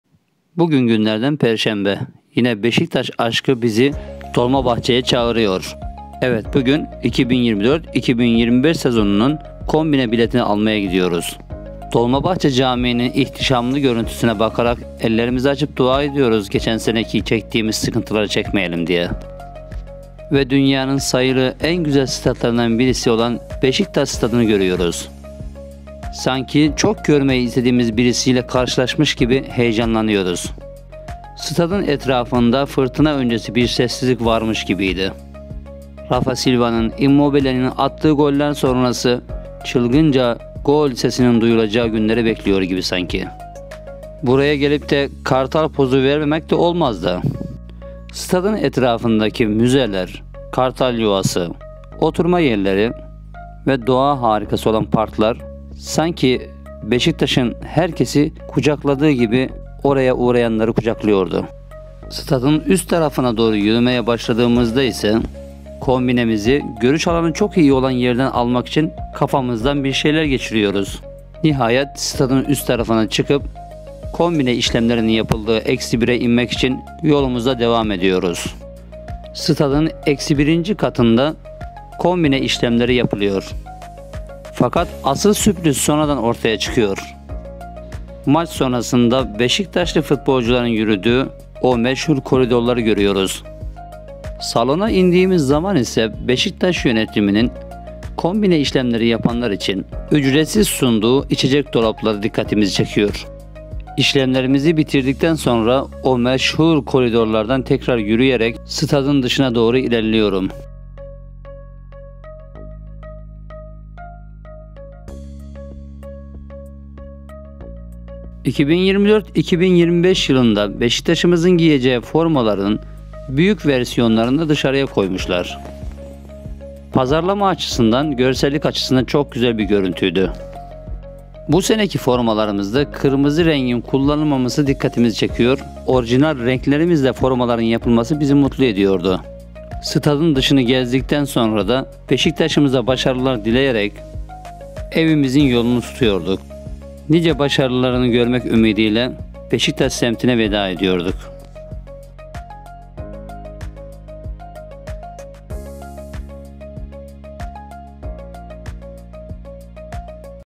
2024-2025 Beşiktaş Kombinemizi Yeniledik!! 🦅 Statta Yaptığımız Gezinti Vilog'u
Beşiktaş ın bu yıl oynayacağı maçların kombine biletlerini aldık. Beşiktaş kulübüne destek olduk. Beşiktaş taraftarı olarak yeni transferleri izleyeceğiz